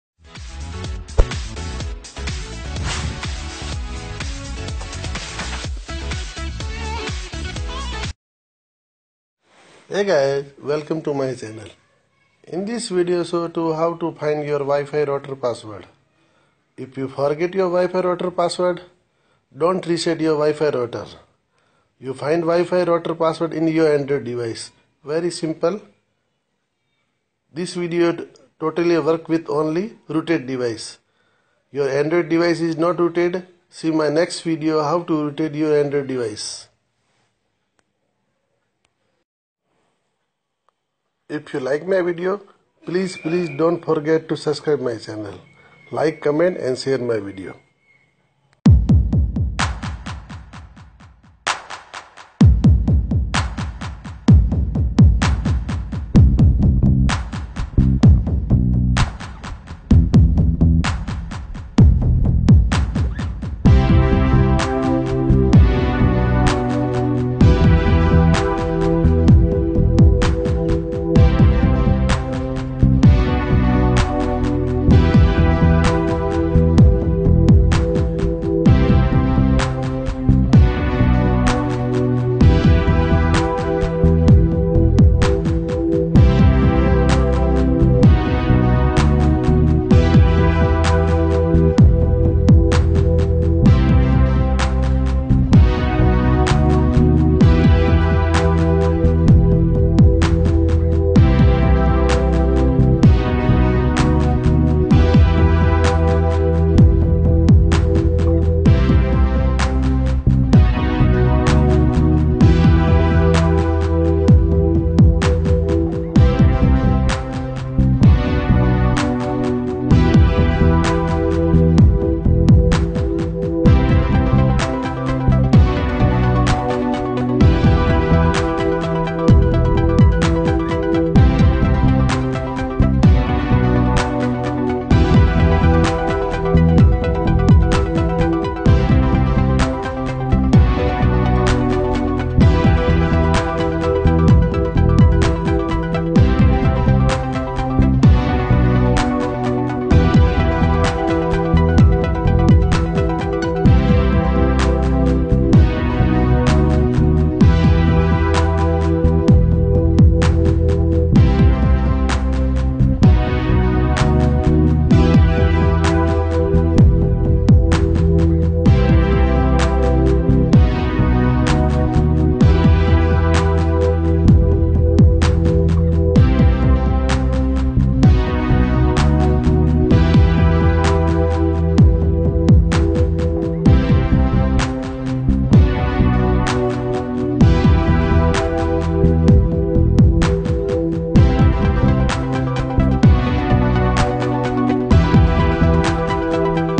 How to hack any wifi | revealed 100 % working method | hack any wifi easily
Hello friends
In my this video i have shown that how to hack any wifi easily so watch  this video and enjoy the hack.
Thank u
Technical kartik